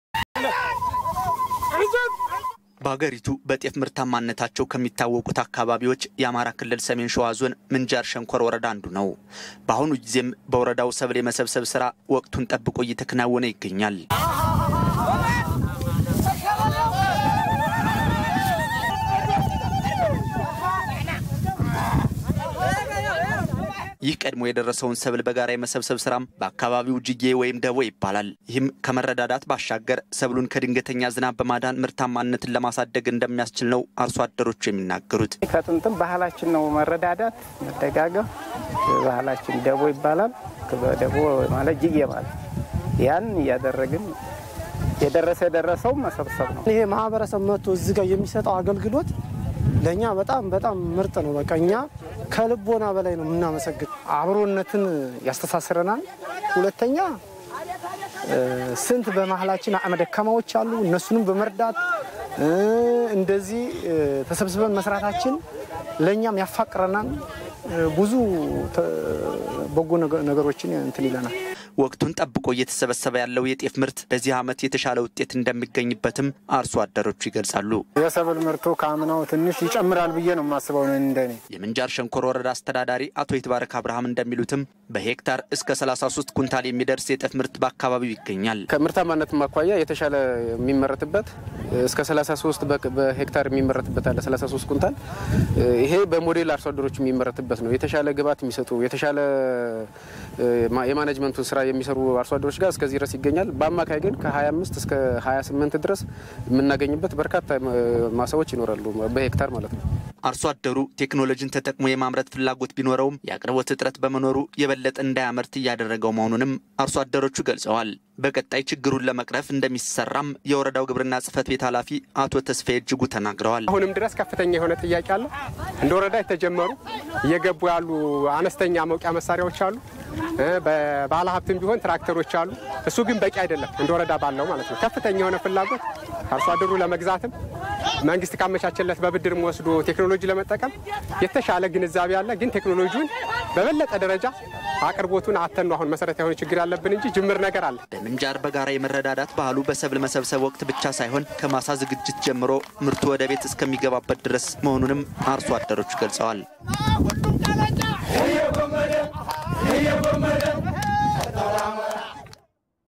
በዚህ ዓመት የተሻለ የጤፍ ምርት ማግኘት እንደሚችሉ በአማራ ክልል ሰሜን ሸዋ ዞን የምንጃር አርሶ አደሮች ገለጹ። | EBC
etv በዚህ ዓመት የተሻለ የጤፍ ምርት ማግኘት እንደሚችሉ በአማራ ክልል ሰሜን ሸዋ ዞን የምንጃር አርሶ አደሮች ገለጹ።